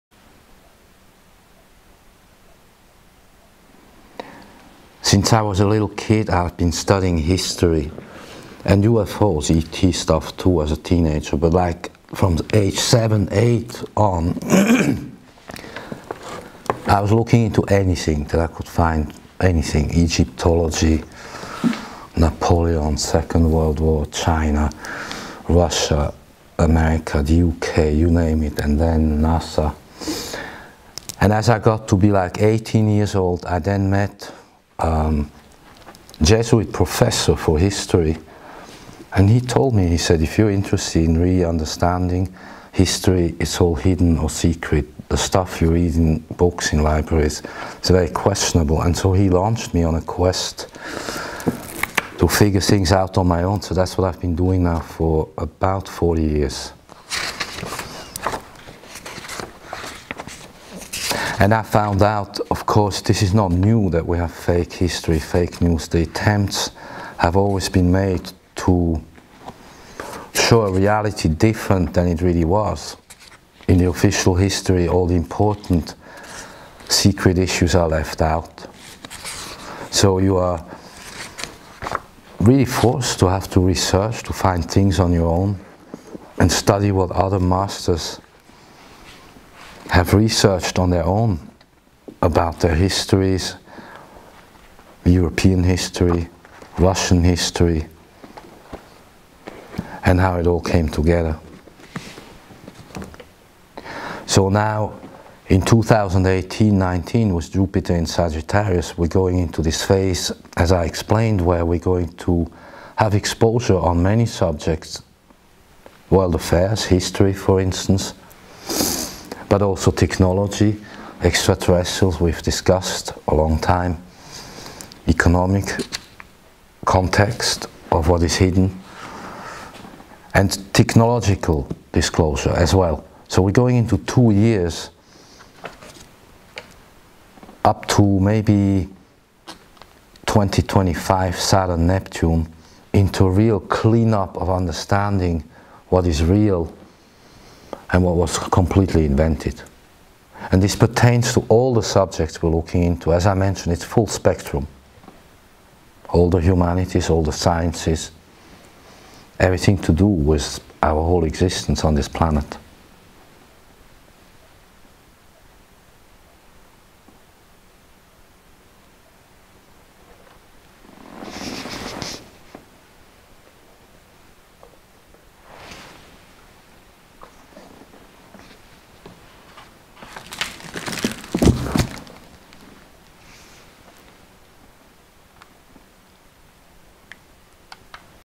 Nordic*Asset - Hidden History Parapolitical
The Little Red Man (of destiny) advised against attacking Russia
inter-dimensional contact being over many centuries.

ET tech transfer exposure in context of Time Travel physics
Visual, telepathic´& hardware contact, plus Future message
Jupiter Sagittarius In 2019 [Issues of Higher Laws]
Saturn in Aquarius 2021 group system management upgrades
TIME MAP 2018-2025 compares sociopolitical + mental climate to current astro-events.

Year of Earth Dog - Earth´change year. Carnival 2018 revealed who + what is behind the fast falling masks.

Now we are entering new discussions & new discoveries. Winter 2021 the swearing in ceremony of the President of the United States we see Saturn & Jupiter in Aquarius & at this point a whole new connected society begins.

St.Clair Talks Astro*Intel: Aquarius Pluto - this is a long term 20 year phenomenon / influence of human's lives on Planet Earth ... "Every 250 years Pluto tears it all apart!" - St.Clair

Filmed in 4K with Panasonic HC-VX11 32x izoom
Saramonic Wireless UwMic9 Dual Microphone